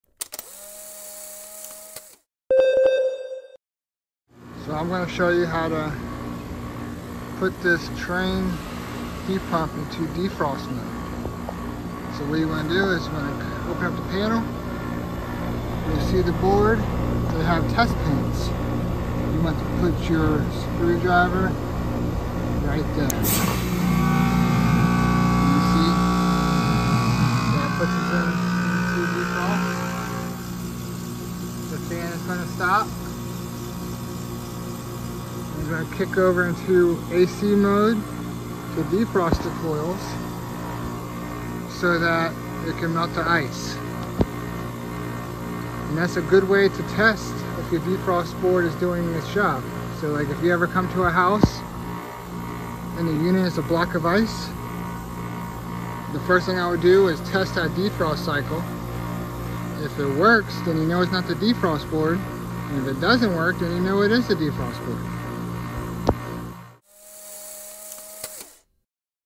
Testing the defrost cycle on a Trane heat pump.
Steps on testing the defrost cycle on a Trane heat pump.